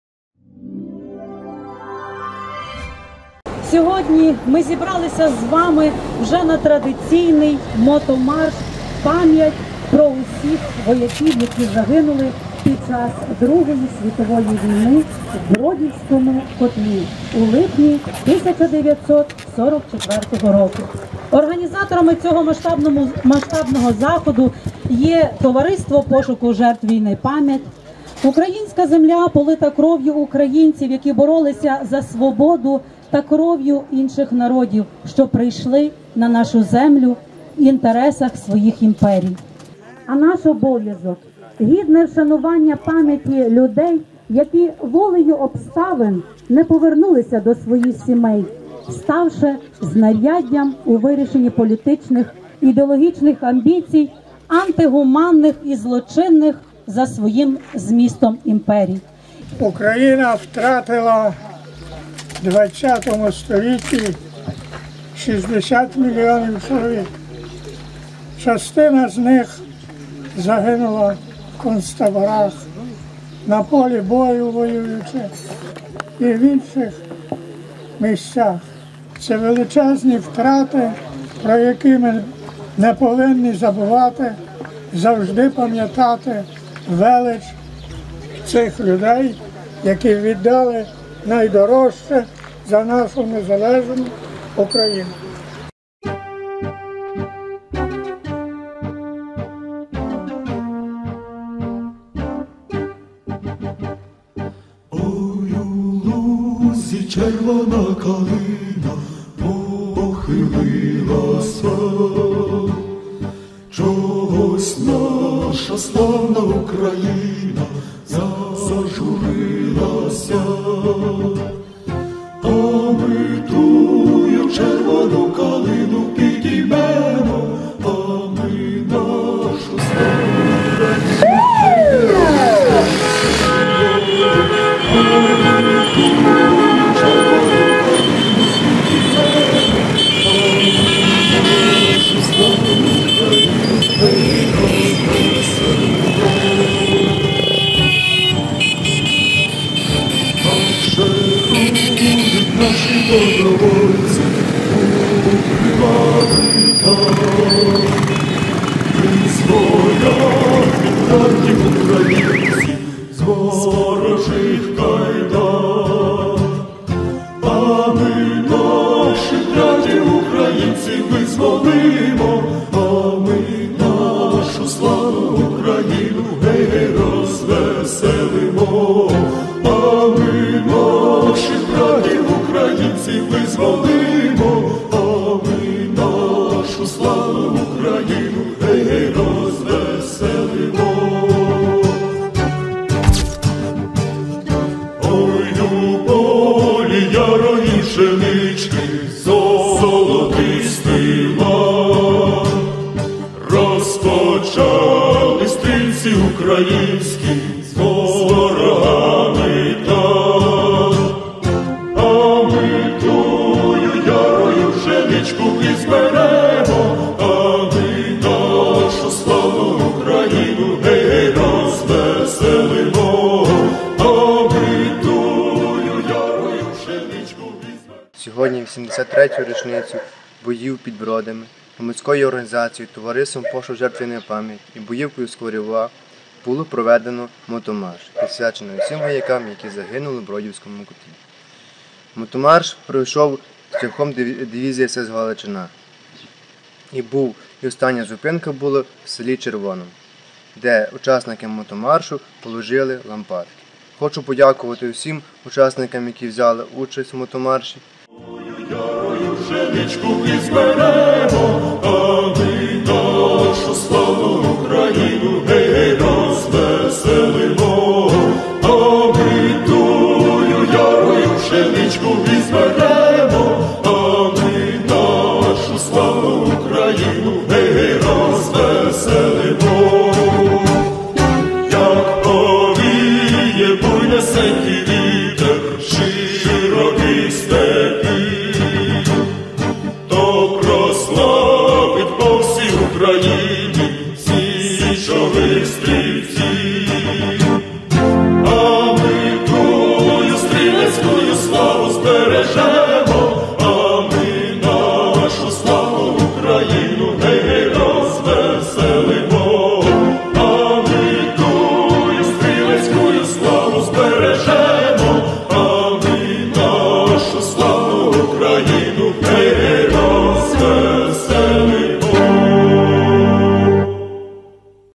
У Золочівському районі відбувся мото-марш
23 липня 2017 року у Золочівському районі відбувся традиційний щорічний мото-марш В ПАМ`ЯТЬ ПРО УСІХ ВОЯКІВ, ЯКІ ЗАГИНУЛИ В “БРОДІВСЬКОМУ КОТЛІ” У ЛИПНІ 1944 РОКУ.

Виїхали учасники маршу із села Скварява, а завершився марш у селі Червоне. Учасники заходу віддавали шану визначним місцям та героям, запалюючи лампадки.

Організували та провели захід Товариство пошуку жертв війни “Пам’ять”, боївка “Скварява”.